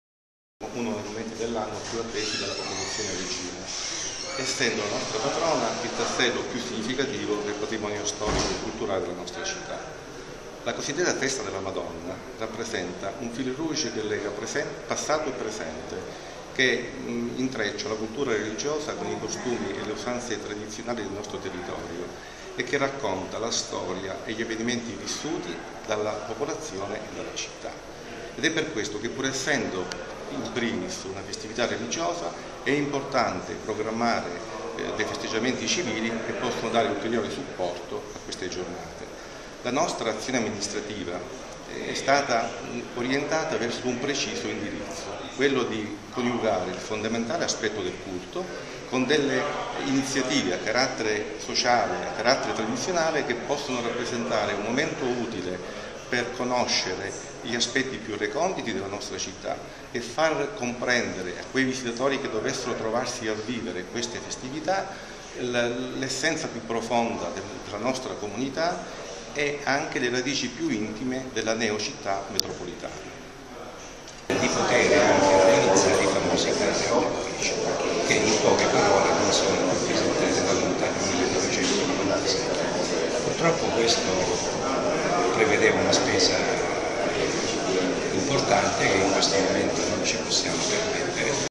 Festività mariane 2012. La tradizione si rinnova a Reggio Calabria
Festività mariane 2012. La tradizione si rinnova a Reggio Calabria. Parla l'assessore alle attività produttive Paolo Anghelone